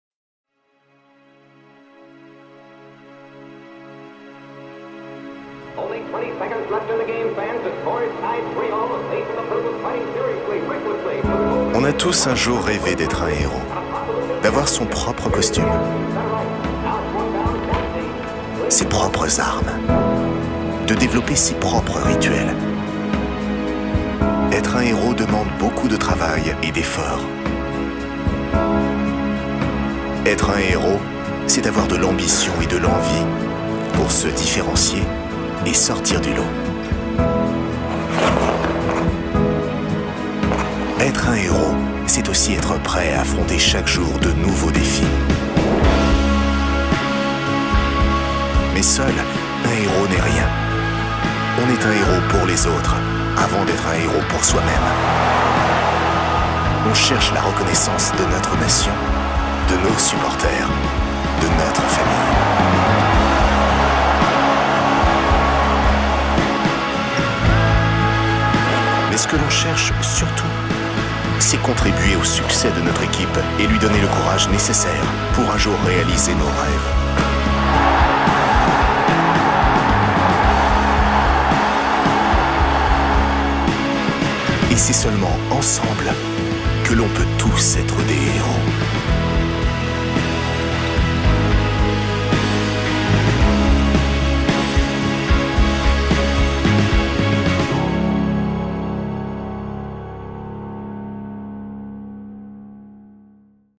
FFHG // WE CAN BE HEROES
Leading up to the 2017 Ice Hockey World Championship, we led a communication campaign to bring people to the 22 games set in Paris, and to raise awareness about this underrated sport in France.